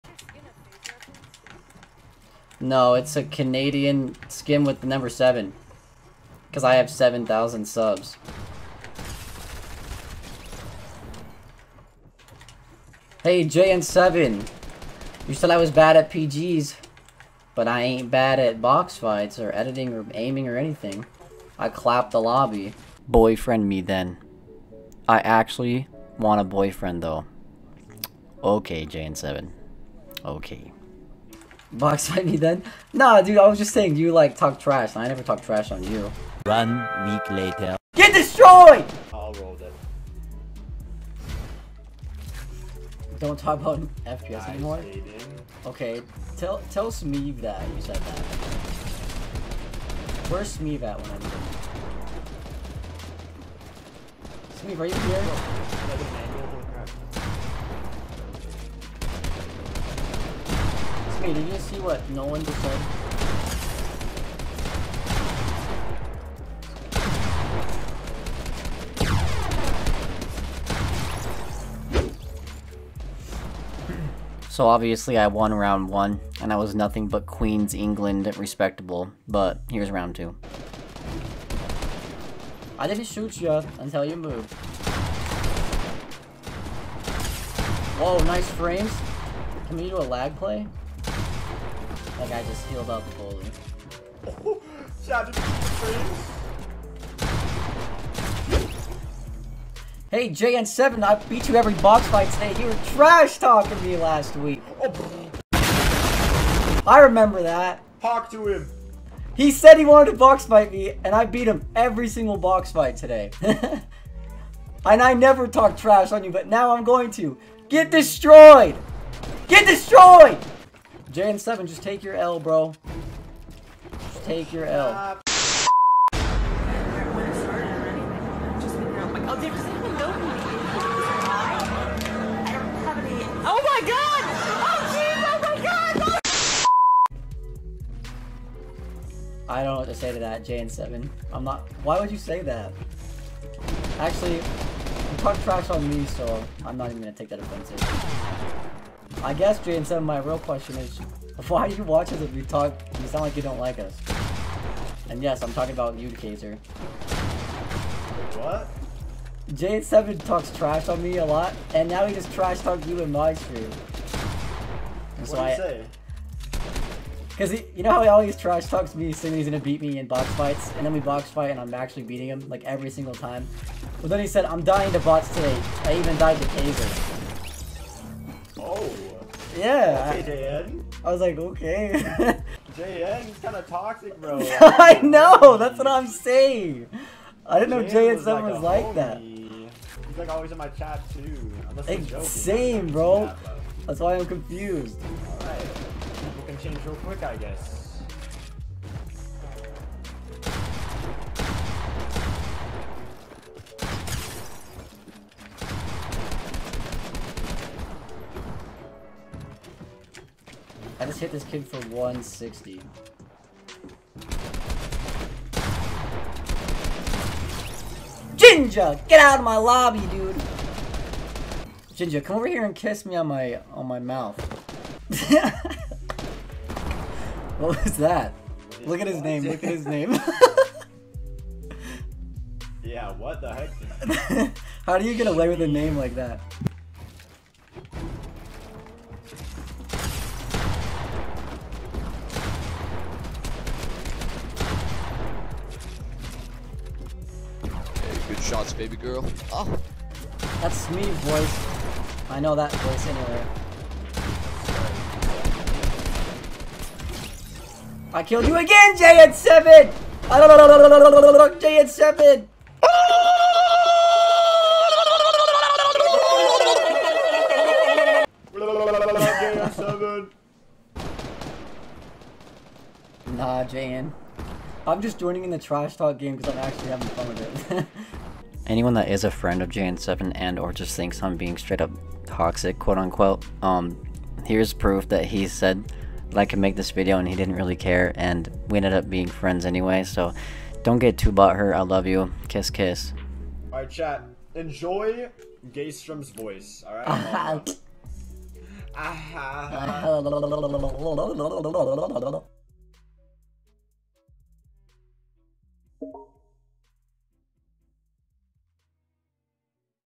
I 1v1 and Absolutely DESTROY a Trash Talker on Nintendo Switch!
Nintendo Switch 1v1 Boxfight. 30fps.
Yes he really was trash talking, it all happened on multiple streams, and it lead to this! lol
I focused today's video on content as well as making the video shorter. I hope to see more feedback from a shorter video! Just testing YouTube algorithm and my subscriber base to see if people would rather watch shorter videos. Anyway these events happen LIVE on twitch

Shoutout to:
thumbnail: Mansa Cacti
trashtalker: Jn7
KayzerMC for being there!

do people even read these lol